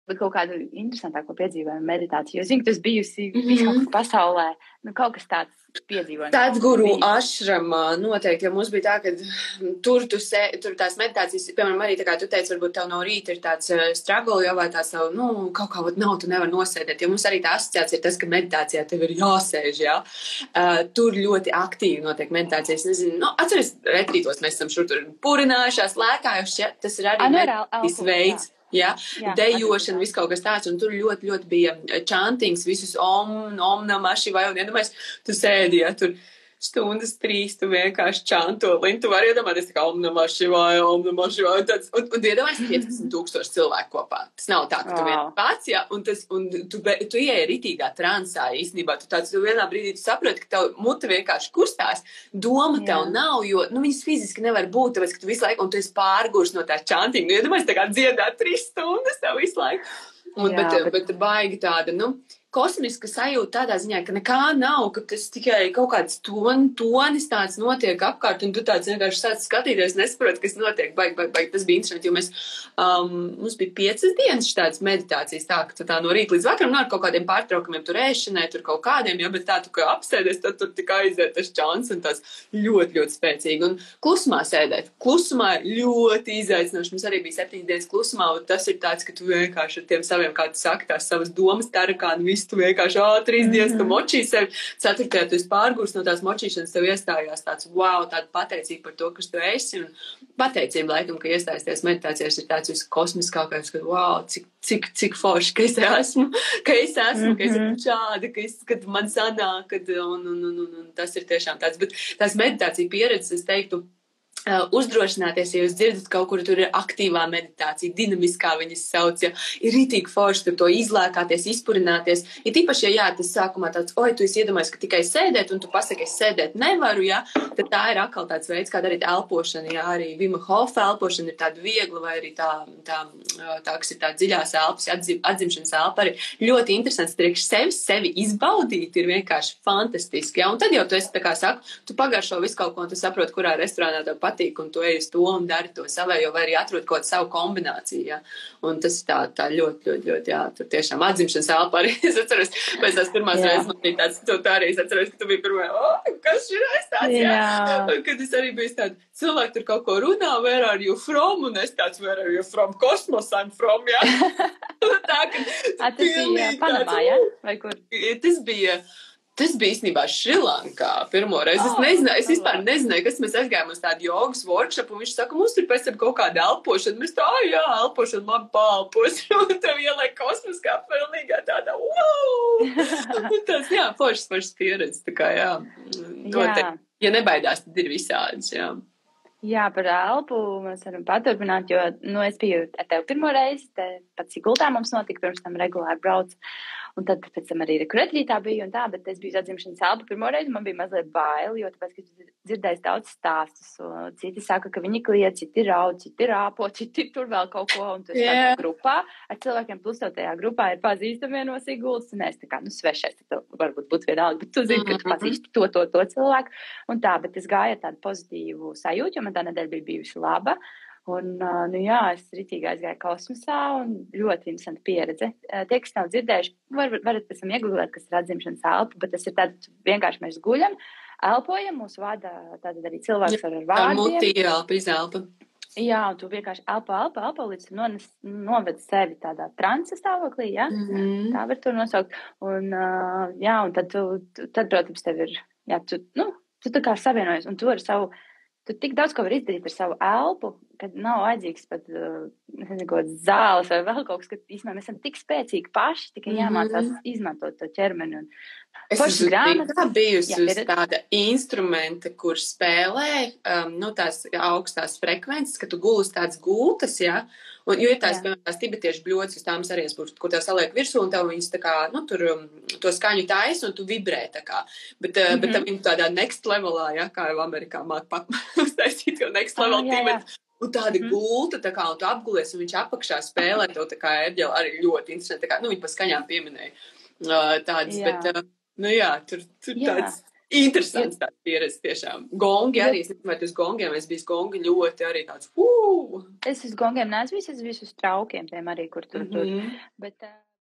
Mans interesantākais piedzīvojums meditācijā!
Stāsts par to, kā es trīs stundas kopā vēl ar piecpadsmit tūkstošiem cilvēku ieplūdu transa kosmiskajā sajūtā.
Un septiņas dienas klusumā - kā tas izmainīja skatījumu par sevi?

JAUNUMS! Podkasts "365 meditācijas katrai dienai"